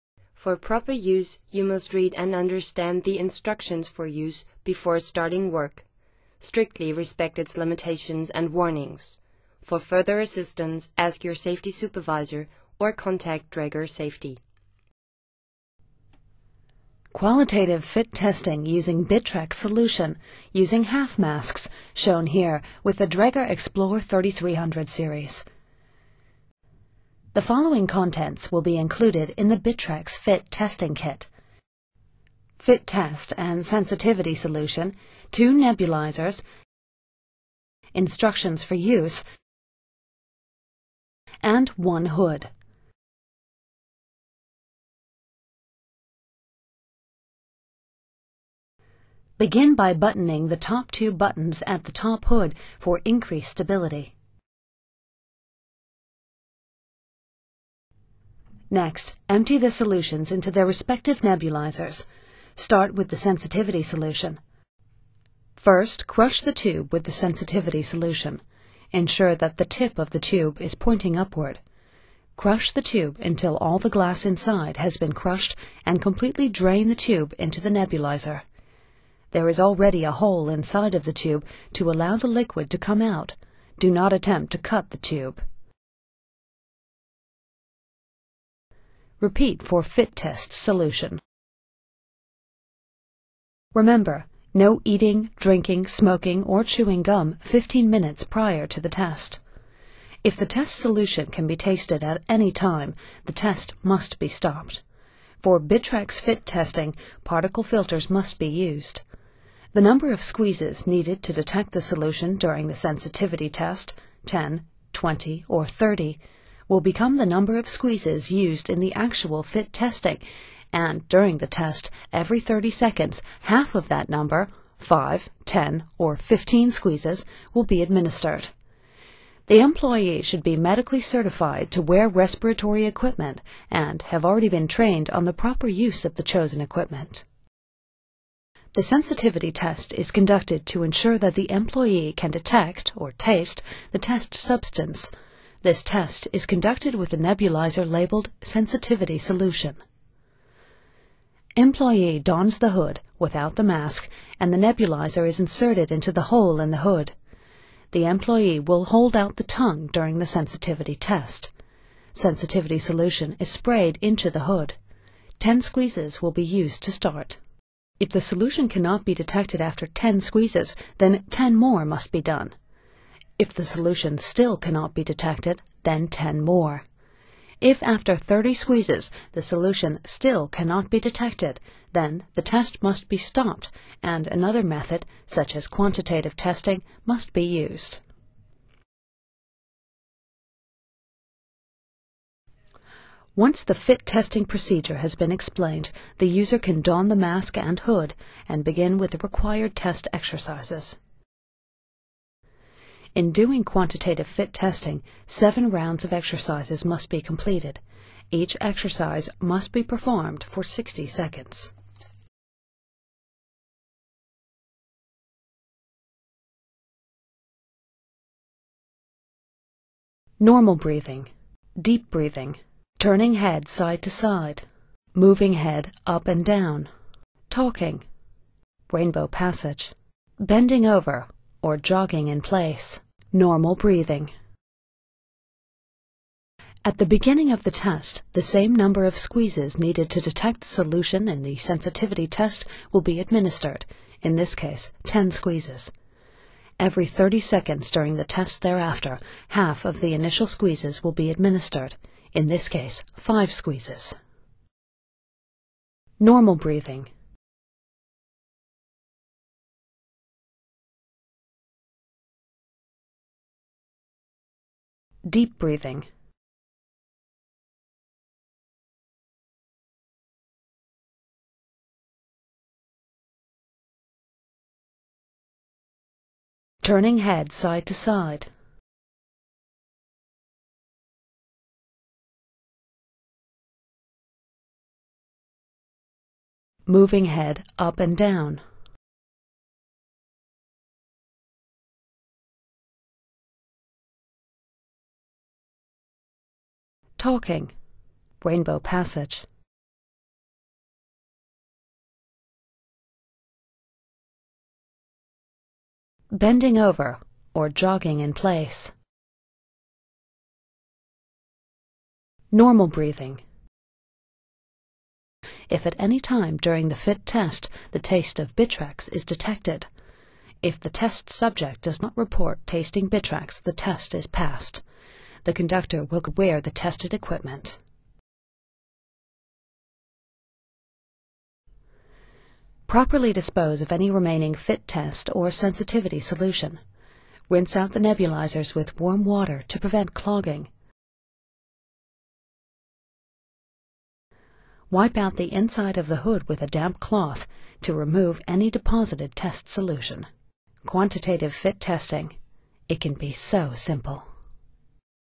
Qualitative Fit-Test Dräger X-plore 3000
A Qualitative fit test checks the fit of a respirator by means of using a test substance that the subject can taste or smell.  The test includes a range of prescribed exercises to be completed, such as breathing normally, breathing deeply, rotating, rearing and bowing the head, bending and talking. This simulates a typical sequence of movements during tasks involved when wearing the respiratory equipment.

More Information on the Dräger X-plore 3000 can be found